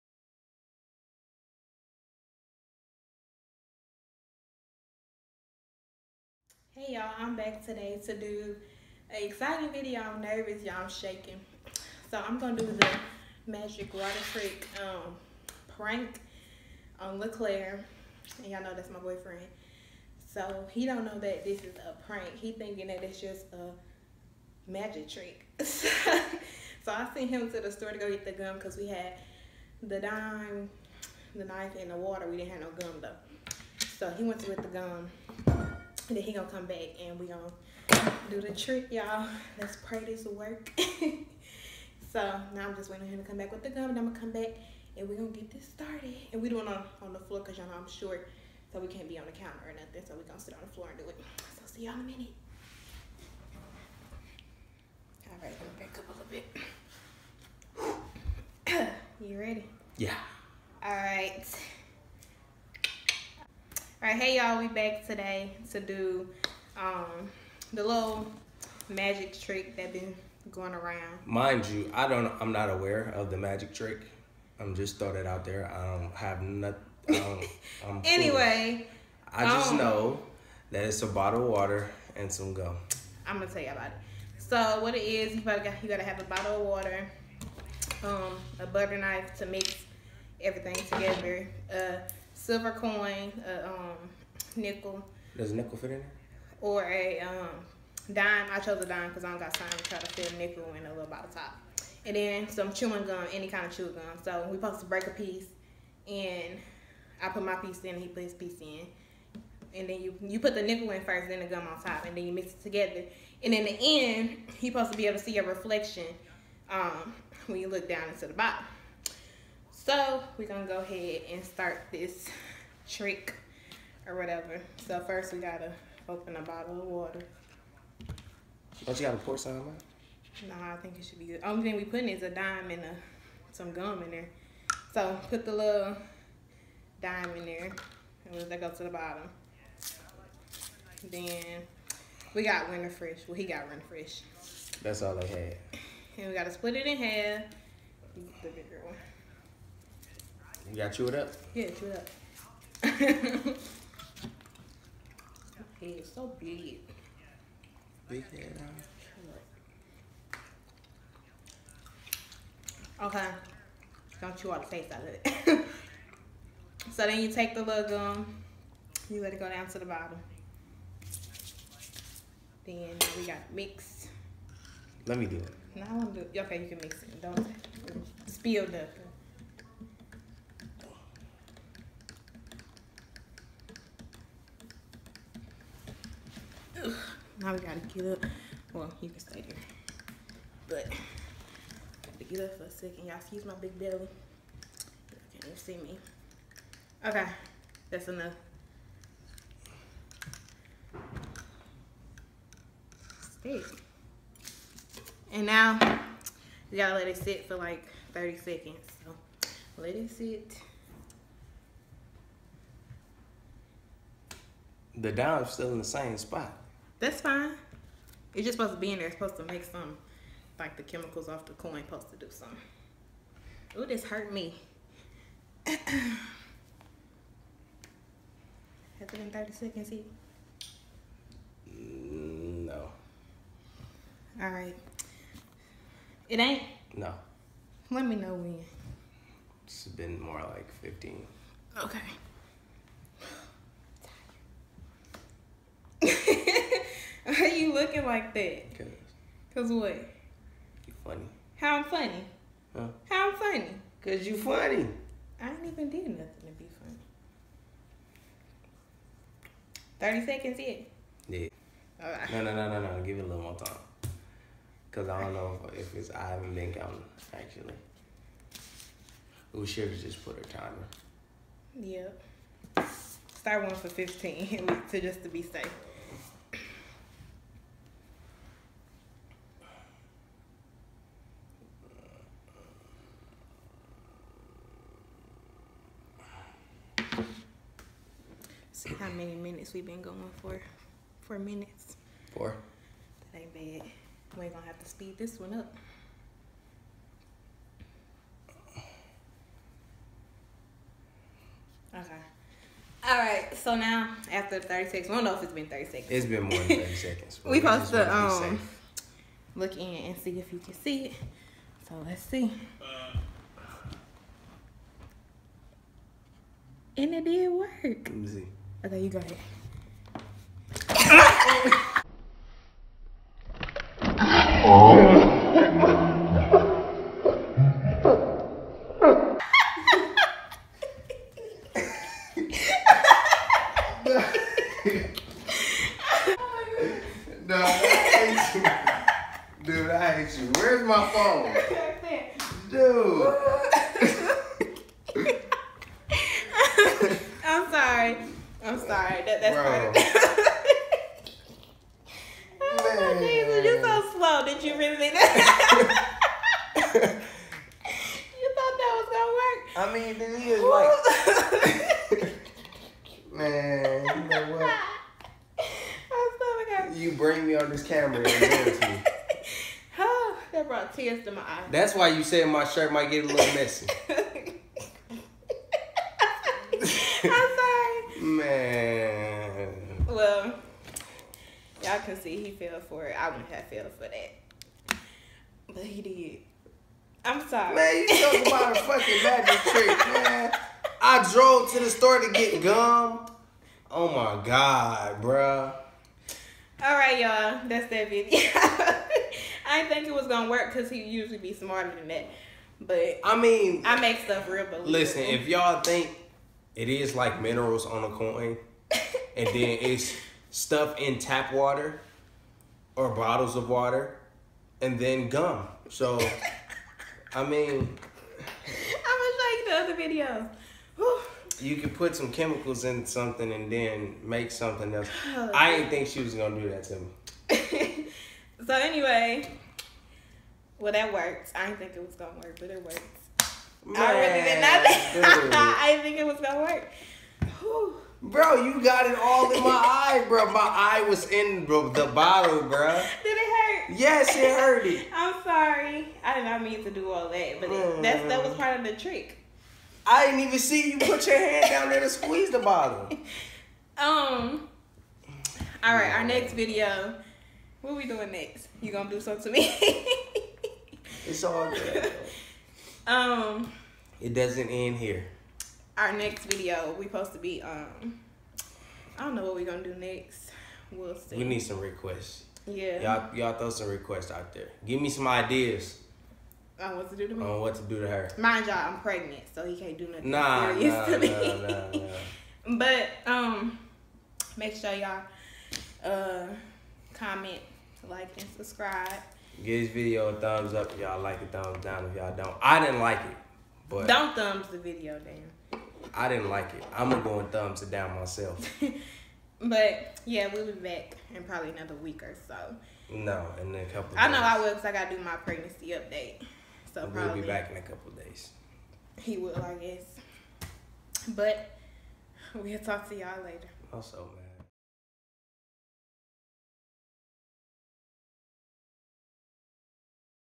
Magic Water Bottle Prank On Boyfriend! (GONE WRONG)
Hey y'all! We back with another video today! This time I prank Le'Clair with the magic Water Bottle trick prank! This was hilarious. Enjoy! Comment any requests down below. What do y'all think I should do to get her back? - Le'Clair

Keep Up W/ Us:

                   Swag_daddy20
                    Bossmanriley
                    LeClair Riley